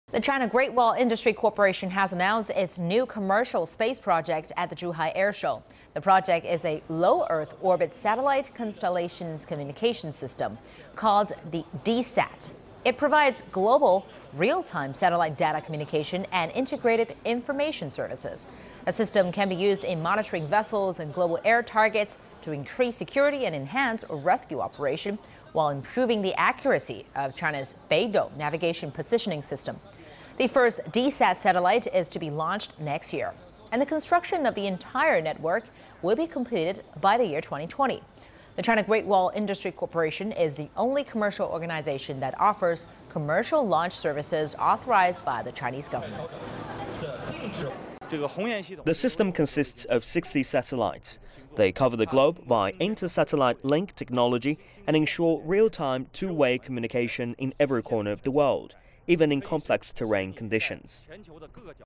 China reveals new space system for global full-time communication
The China Great Wall Industry Corporation has announced its new commercial space project at the Zhuhai airshow. The project is a "Low Earth Orbit Satellite Constellation Communication System", called DESat. It provides global real-time satellite data communication and integrated information services.